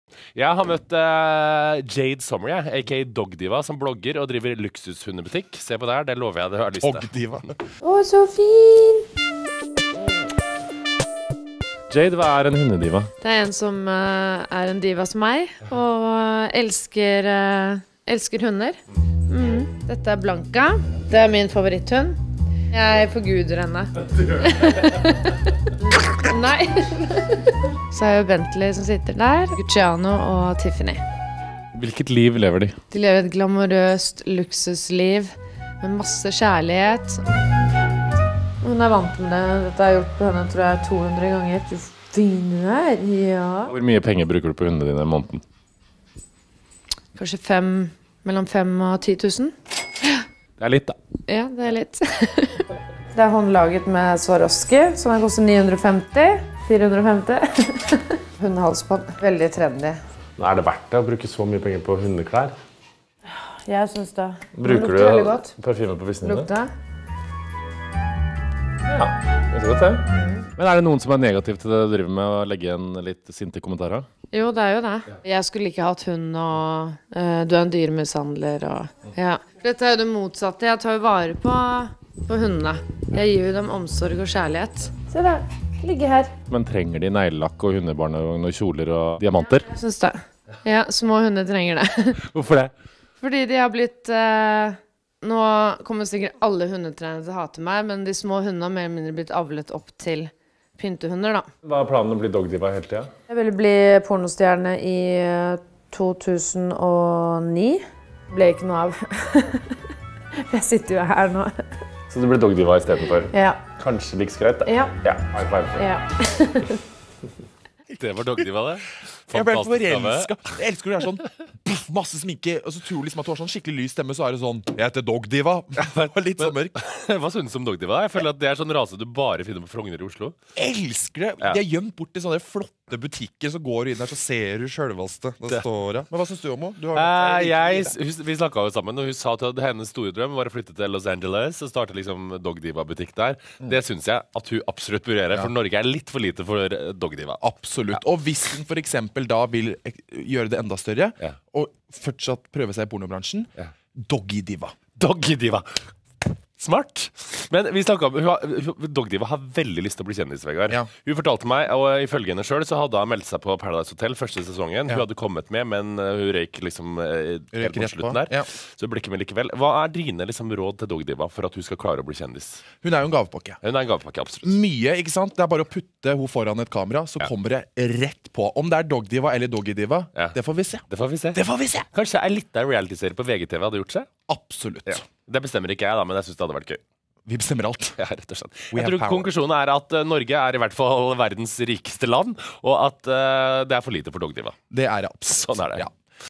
«Dogdiva»: - Hunder trenger neglelakk og diamanter
Den norske hundebloggeren Jade-Madelen Sommer vekker oppsikt.

Denne videoen er tillatt for alle.